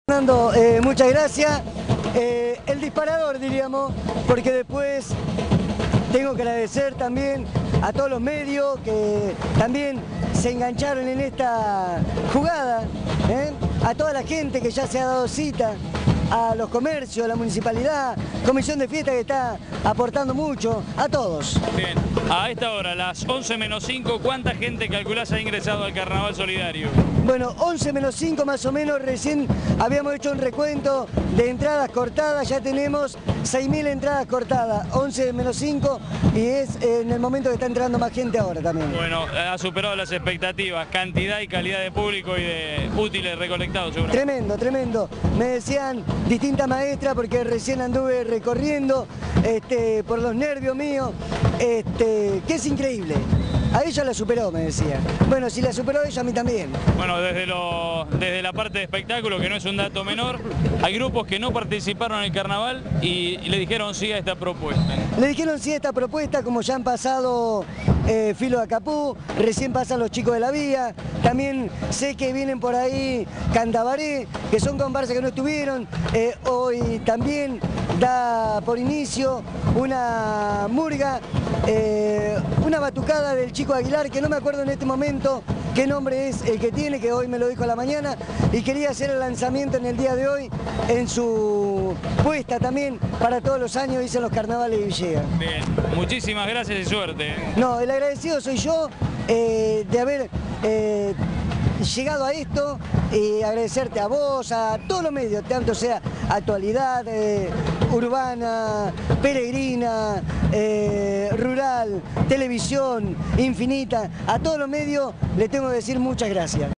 Carnavales solidarios - Villegas Digital
Nelson "Cuita" Surbano, realizó un balance del carnala solidario pr utiles escolares para las escelas de General Villegas.